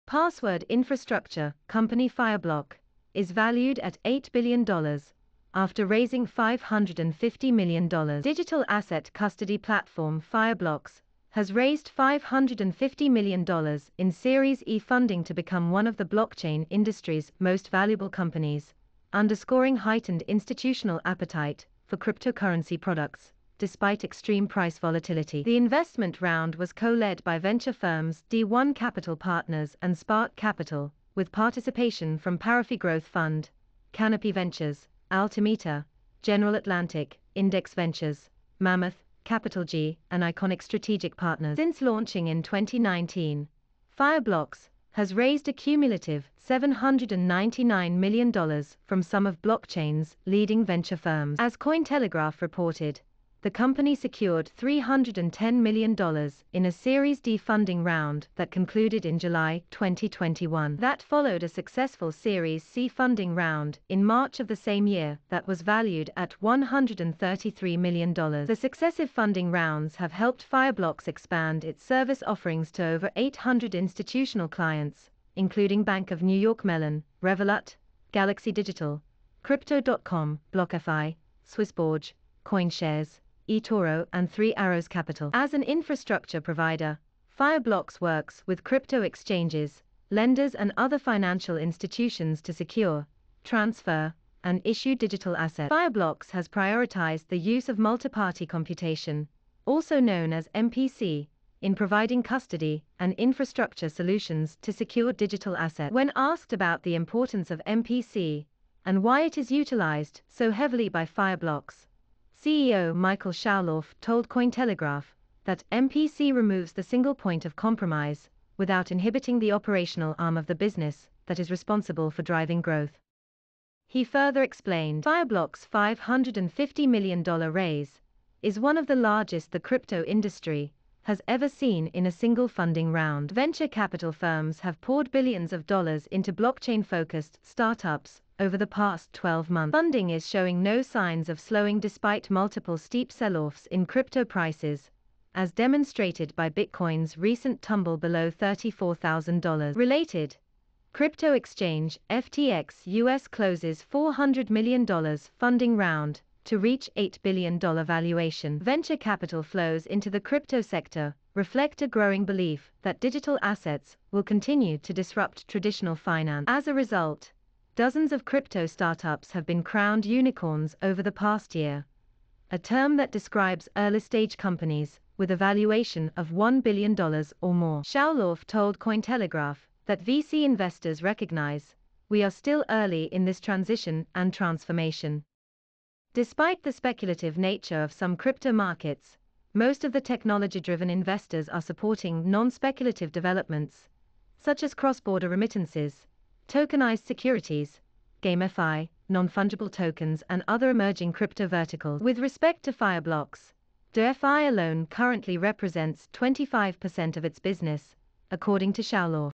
Password infrastructure company Fireblock is valued at $8 billion after raising $550 million
Fireblocks has raised a cumulative $799 million from some of blockchain’s leading venture firms. As Cointelegraph reported Password infrastructure company Fireblock is valued at $8 billion after raising $550 million Digital asset custody platform Fireblocks has raised $550 million in Series E funding to become one of the blockchain industry’s most valuable companies Crypto exchange FTX US closes $400M funding round to reach $8B valuation Venture capital flows into the crypto sector reflect a growing belief that digital assets will continue to disrupt traditional finance. As a result over the past year — a term that describes early-stage companies with a valuation of $1 billion or more. Shaulov told Cointelegraph that VC investors recognize "we are still early in this transition and transformation." Despite the speculative nature of some crypto markets in providing custody and infrastructure solutions to secure digital assets. When asked about the importance of MPC and why it is utilized so heavily by Fireblocks the company secured $310 million in a Series D funding round that concluded in July 2021. That followed a successful Series C funding round in March of the same year that was valued at $133 million. The successive funding rounds have helped Fireblocks expand its service offerings to over 800 institutional clients Fireblocks' $550 million raise is one of the largest the crypto industry has ever seen in a single funding round. Venture capital firms have poured billions of dollars into blockchain-focused startups over the past 12 months. Funding is showing no signs of slowing despite multiple steep selloffs in crypto prices Fireblocks works with crypto exchanges CEO Michael Shaulov told Cointelegraph that "MPC removes the single point of compromise without inhibiting the operational arm of the business that is responsible for driving growth." He further explained nonfungible tokens and other emerging crypto verticals. With respect to Fireblocks